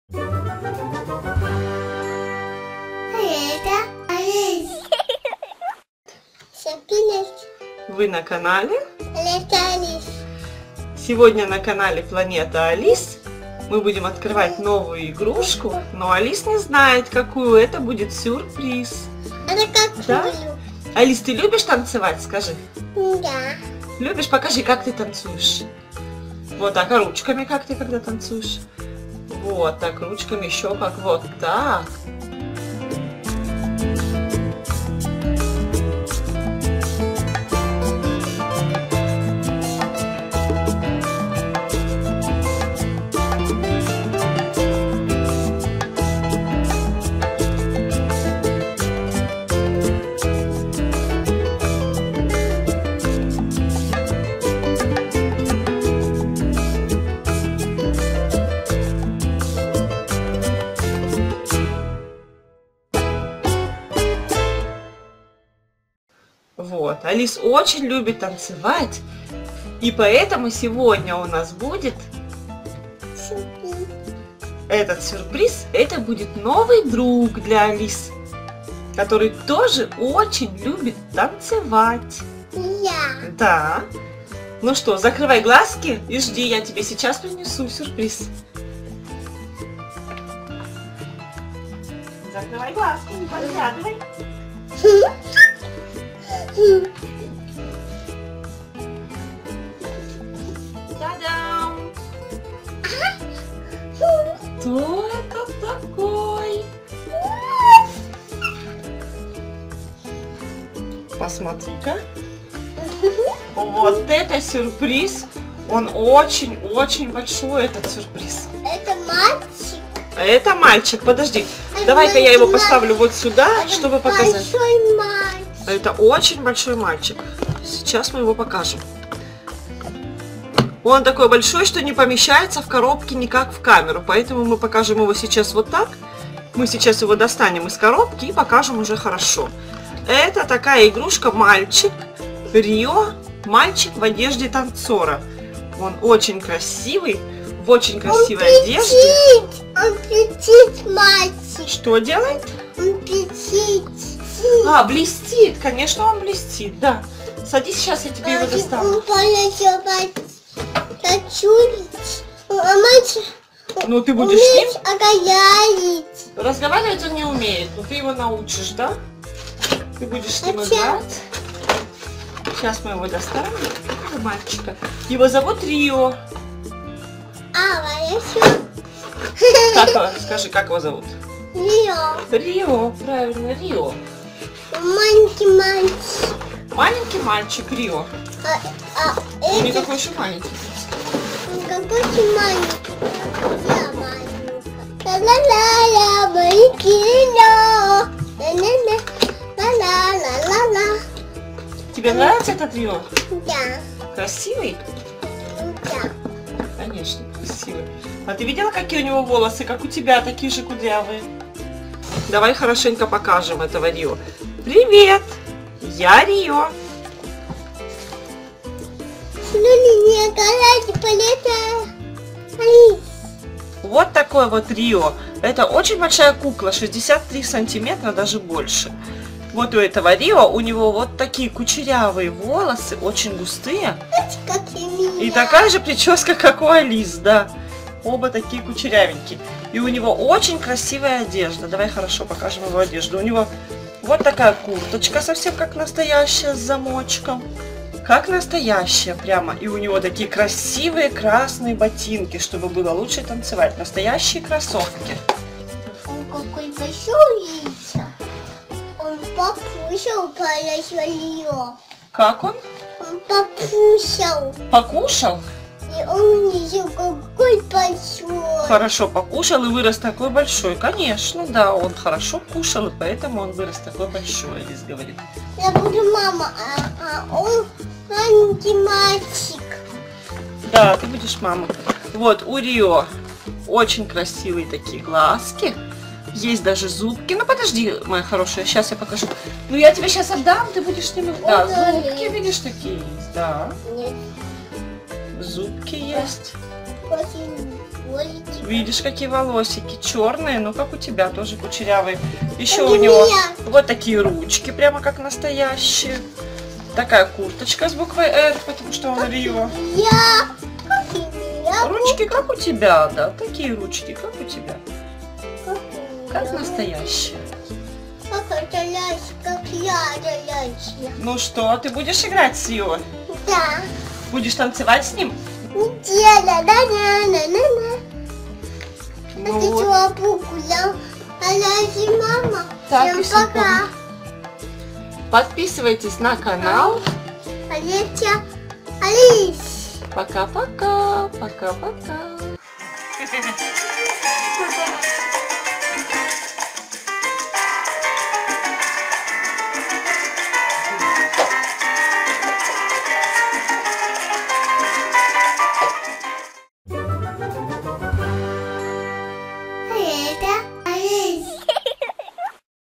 💃Кукла Рио - мальчик в одежде танцора диско:)
Алис получает подарок-сюрприз - большую куклу Рио в одежде танцора. У Алис еще не было таких больших кукол, она в восторге:)

Alice gets a surprise gift - a large doll clothes dancer in Rio. Alice was not yet such a large doll, she is thrilled :)...